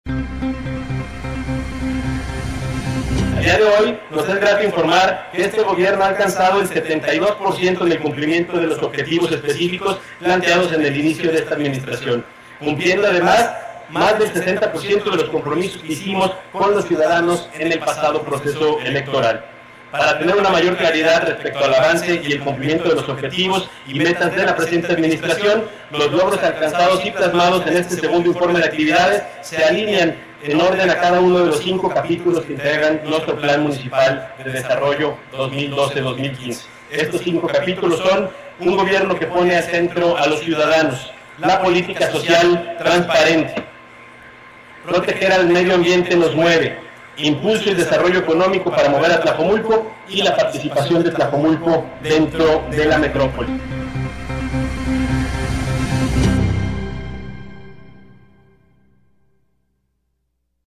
“Se han invertido 173 mdp en tratamiento de aguas residuales en Tlajomulco”
El alcalde Ismael del Toro presenta su segundo informe de Gobierno; pide que no se politice el tema de la Laguna de Cajititlán